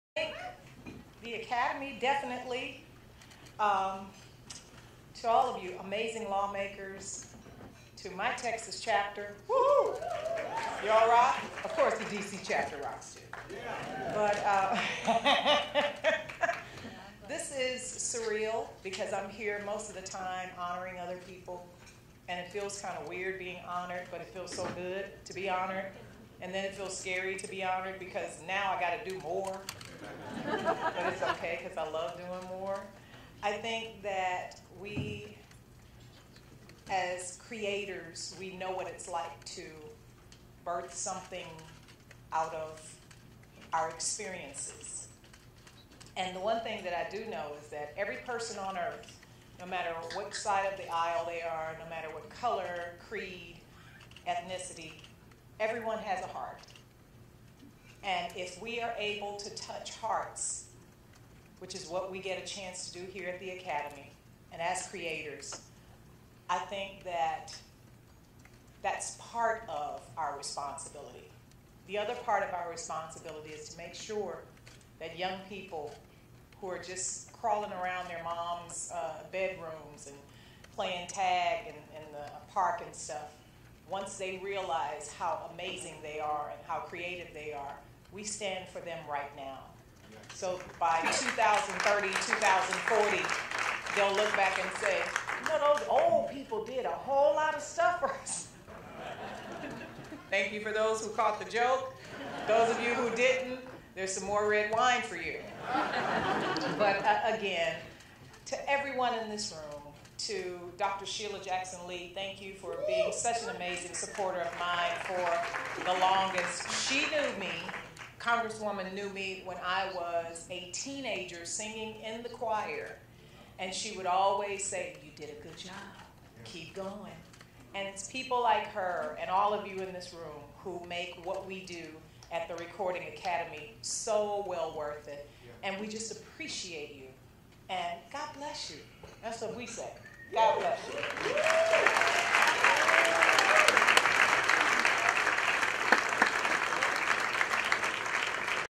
Yolanda Adams' Grammys On The Hill Acceptance Speech
What an honor!! Thanks so much to the Recording Academy and Grammys On The Hill!!